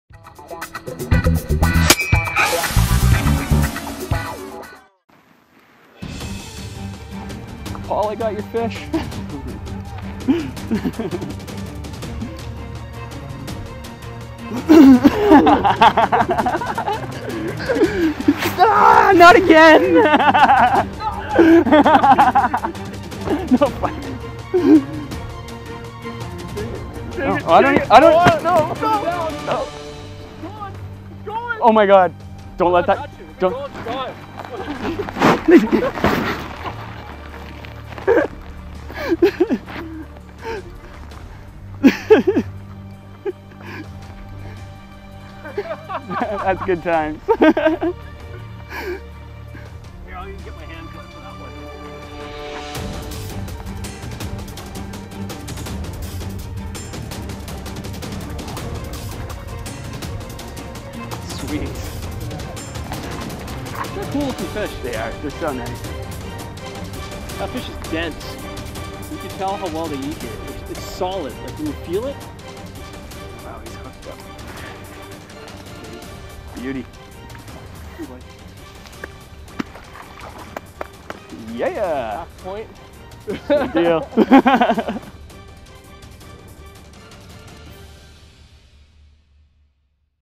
CG Raw Footage : Ice fishing on Lake Simcoe wit Ryan and Paul
Day in and day out, our Crew is on the water doing what they love and having a good time. We just wanted to share a bit of raw footage of Paul Castellano and Ryan Hare having some fun and busting some chops in the process. They still come out of it with a nice Lake Simcoe Trout